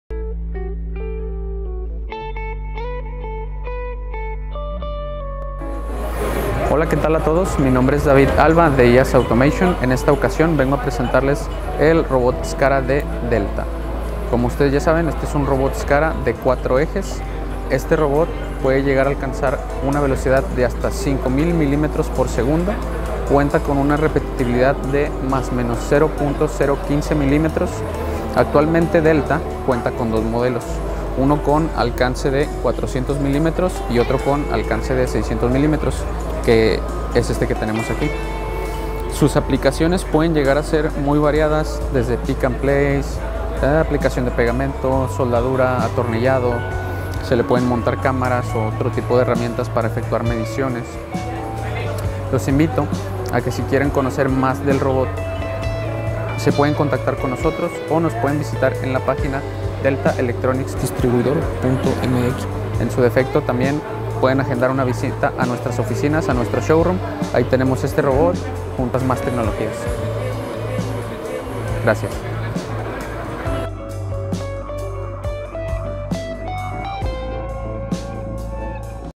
Robot Scara Delta Características y Aplicaciones
El Robot Scara de Delta es un robot industrial que te permitirá realizar tareas de manera eficiente e inteligente.
Conoce más aspectos técnicos en el siguiente vídeo.